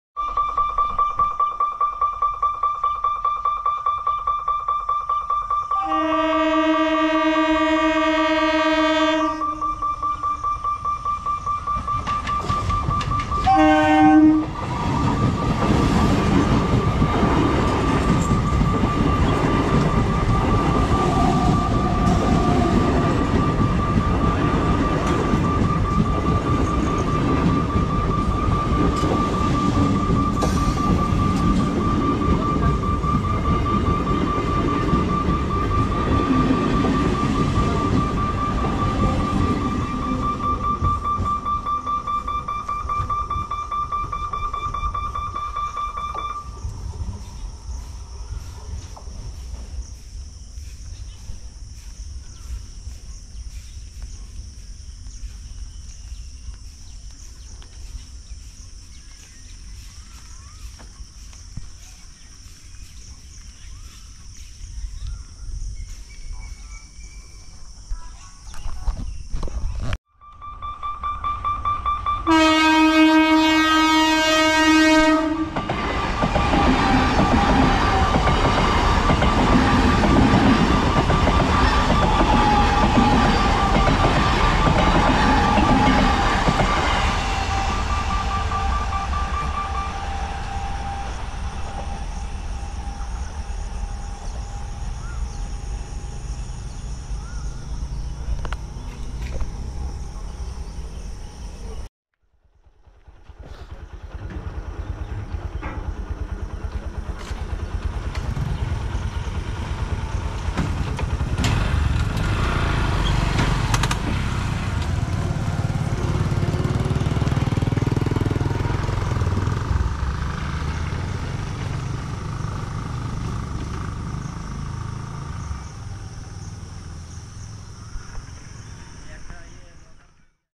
women stuck with cycle old model and new model EMU Train crosses back to back between a ER railgate😯
this video is about  women stuck with cycle old model and new model EMU Train crosses back to back between a ER railgate😯